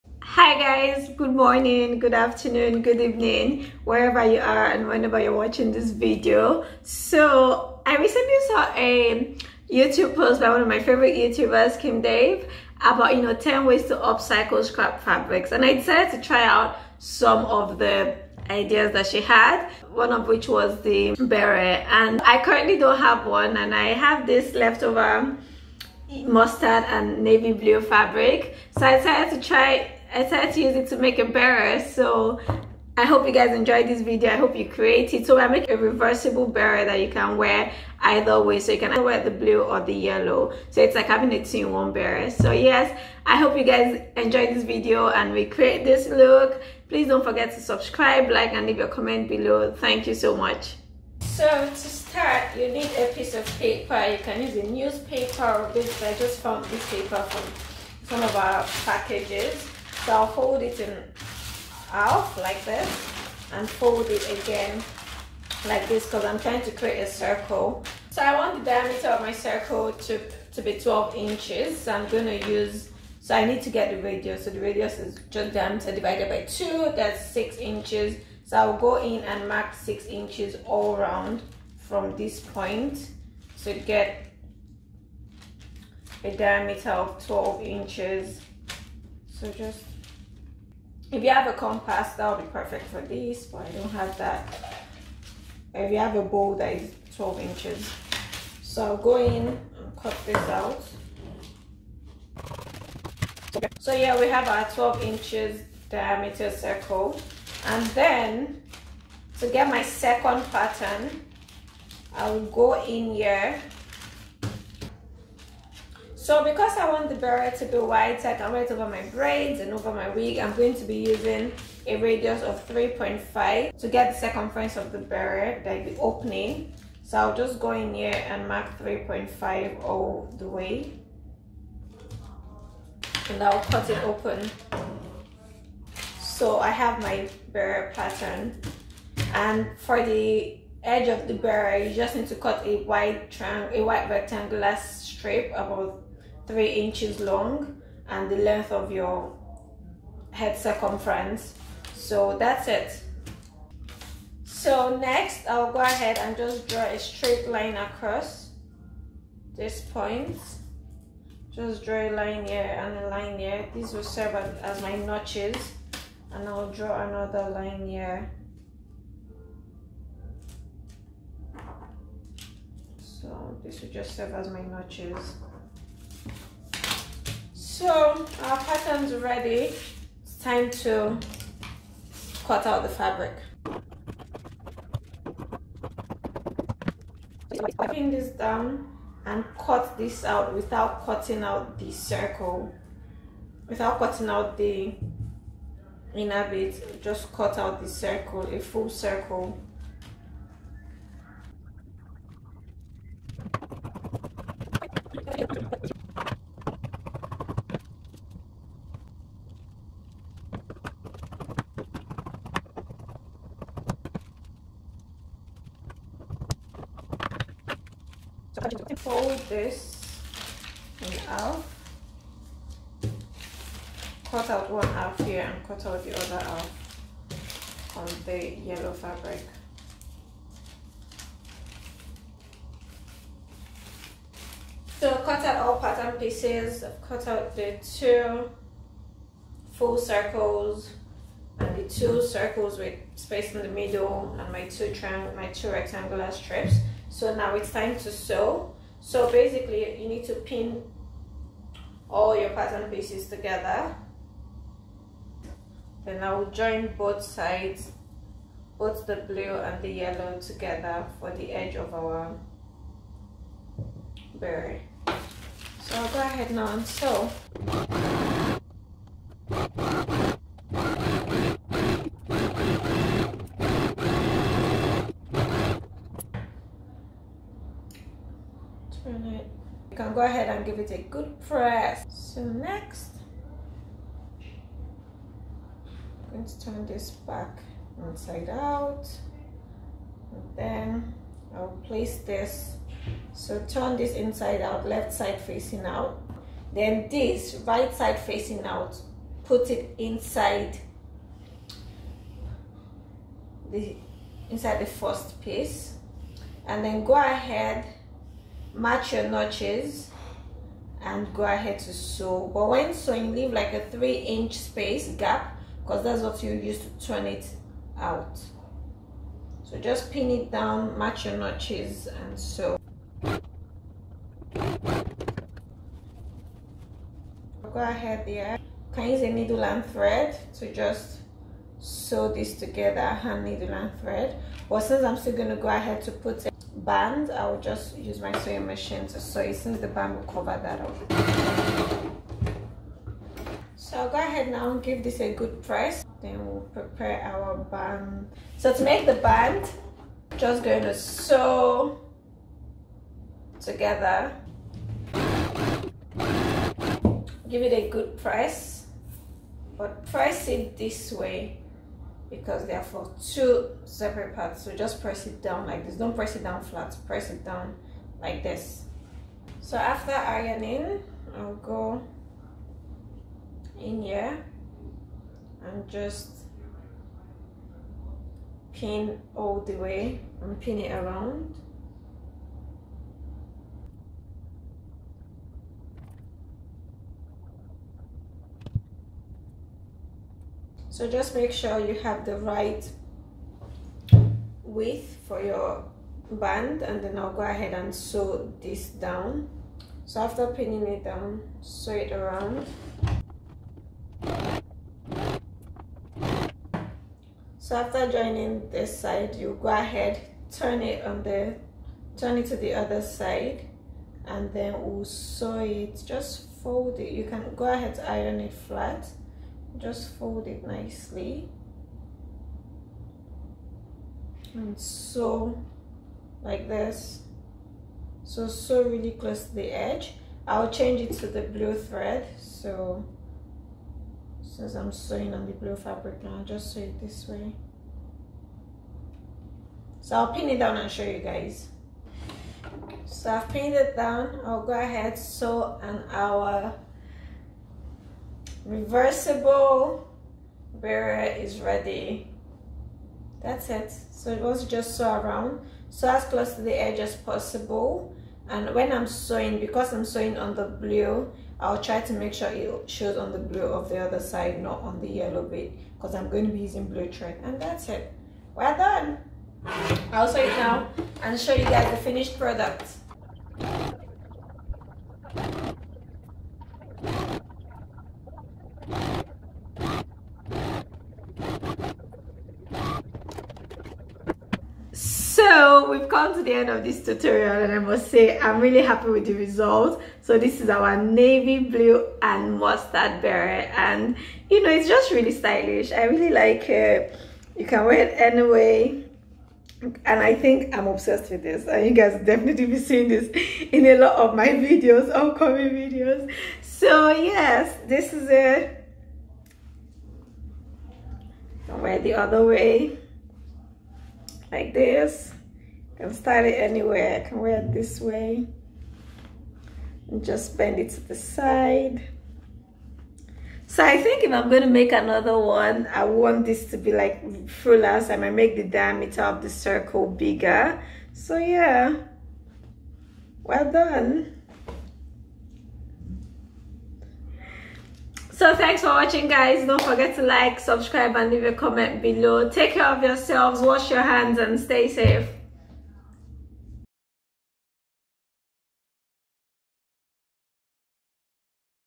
DIY Reversible Fabric Beret|| Detailed and Beginner Friendly
Learn how to cut and sew a reversible fabric beret without any prior knowledge of sewing from scrap fabrics in this video. Beginner friendly and detailed tutorial. Let's work together to reduce waist generated from sewing by up-cycling scrap fabrics. :d

Instagram: @remijunaid (i follow back ;))